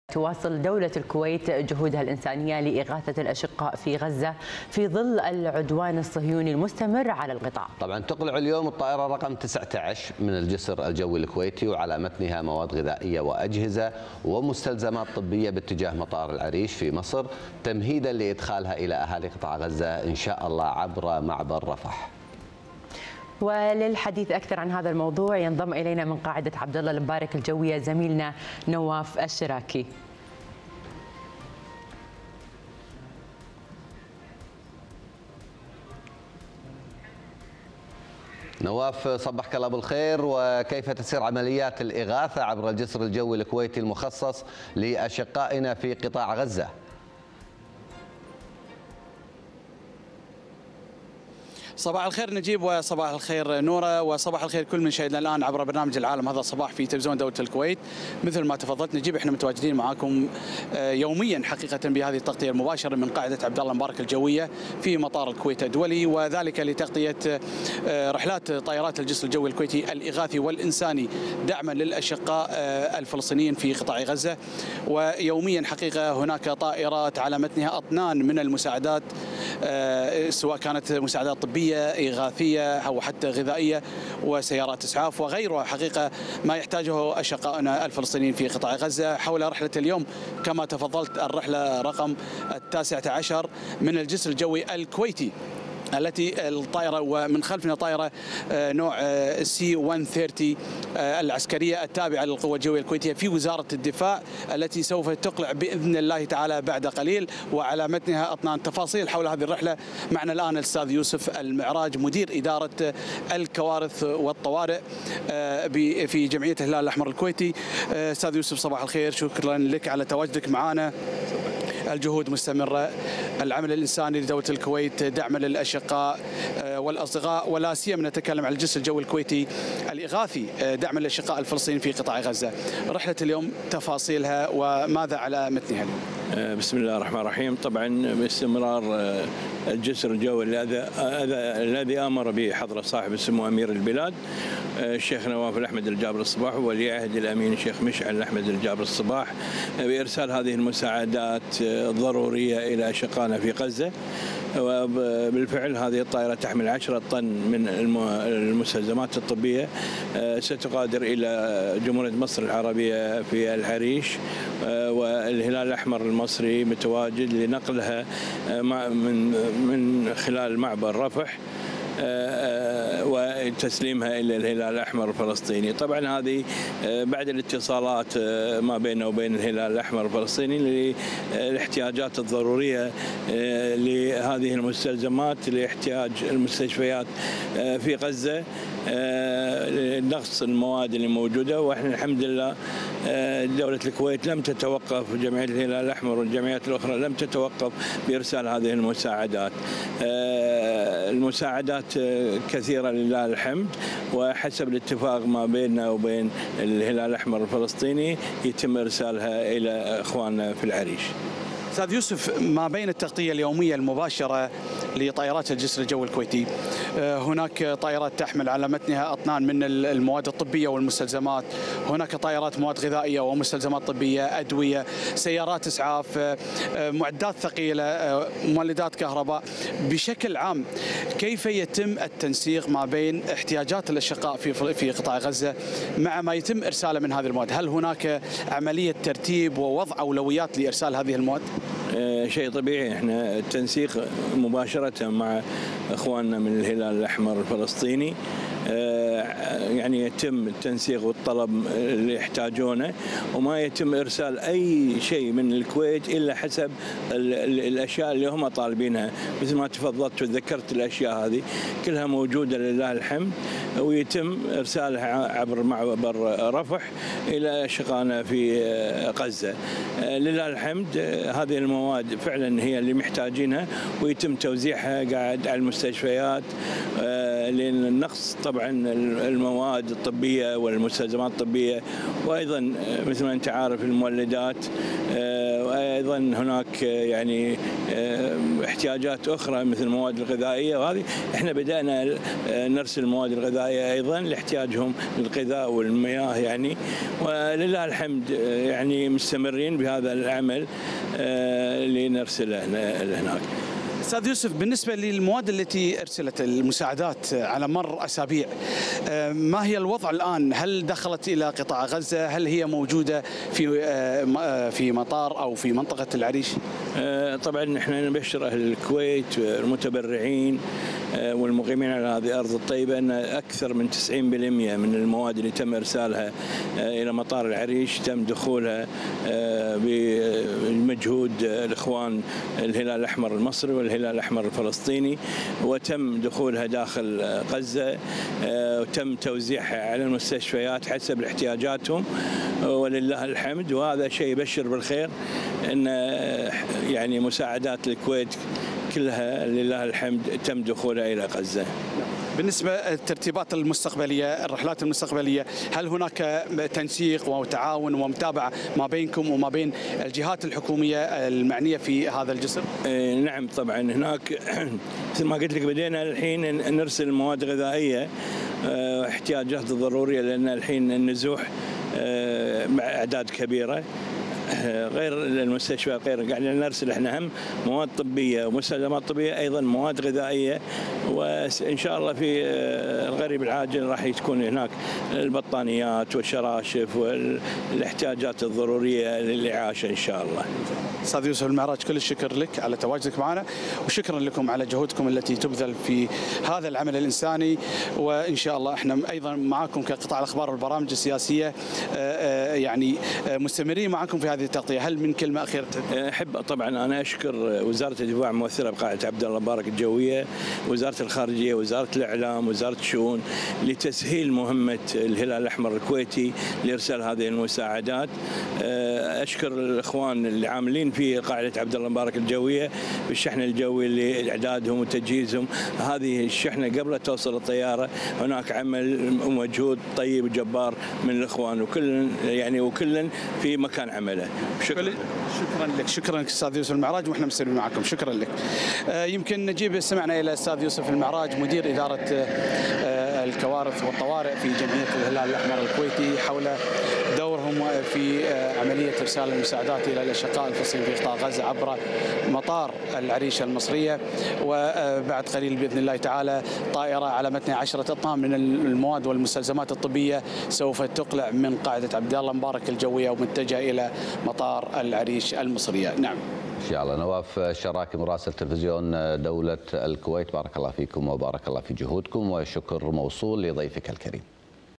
الجسر الجوي الإغاثي الكويتي مستمر في إيصال المساعدات لأشقائنا في غزة
- يوسف المعراج - مدير إدارة الكوارث والطوارئ بجمعية الهلال الأحمر الكويتي